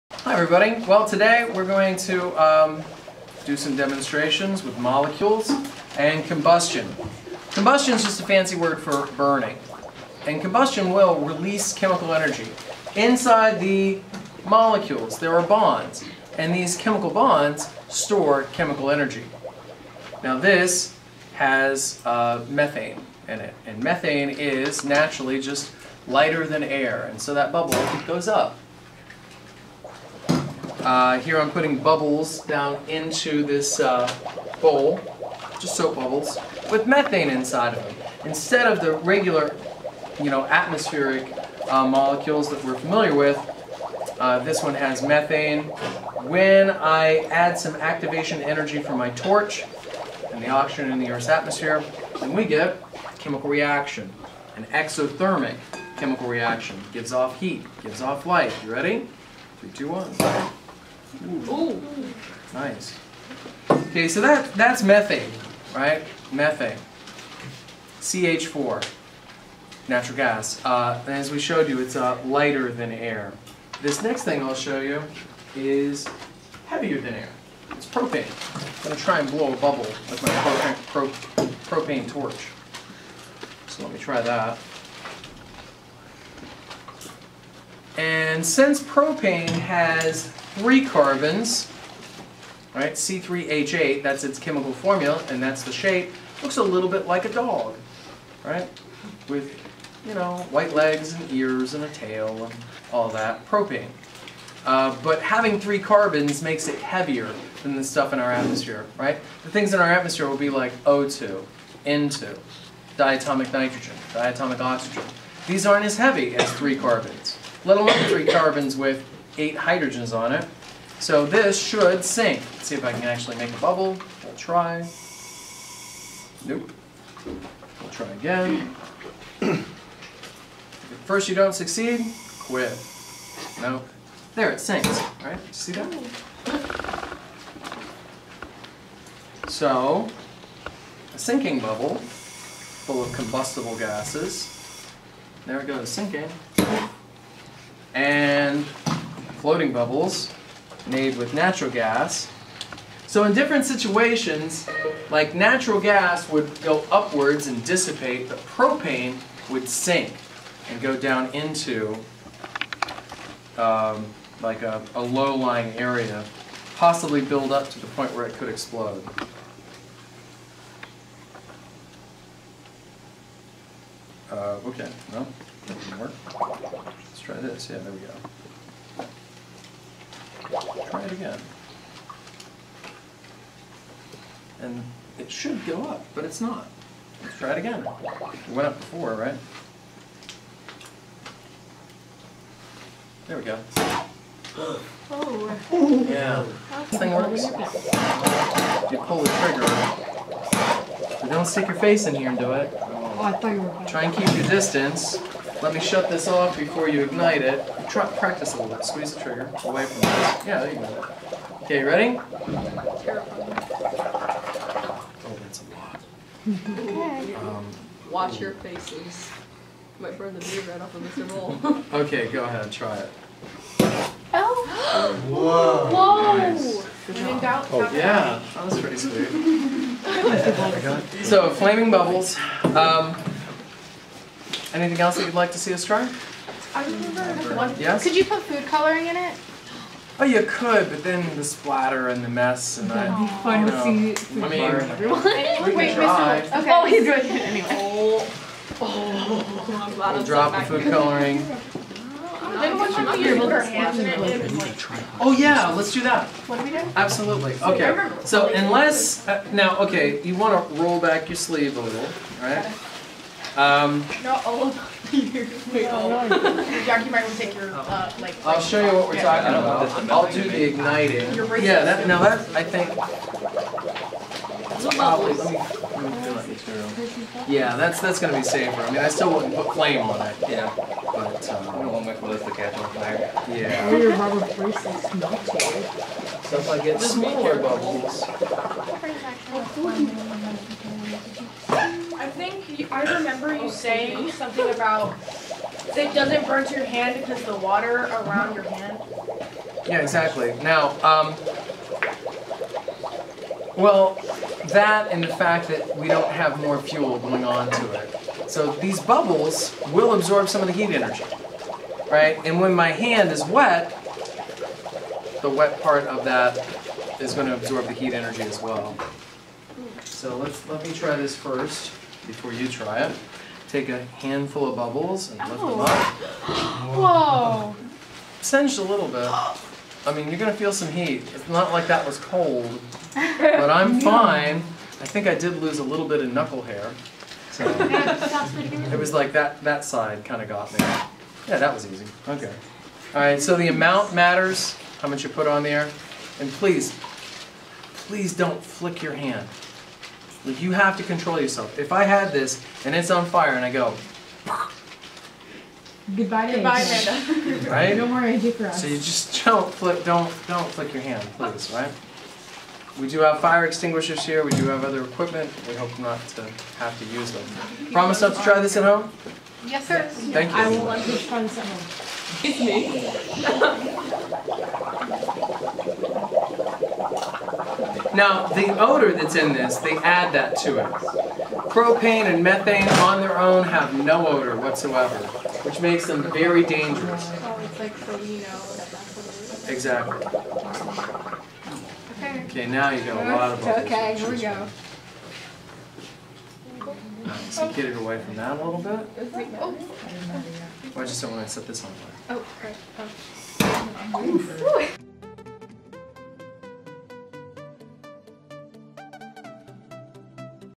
Flaming Bubbles = Combust-a-bubbles?
Mr. Boal shows his ecology club students how bubbles can hold combustible gases and be lit in your hand. Please don't try this at home.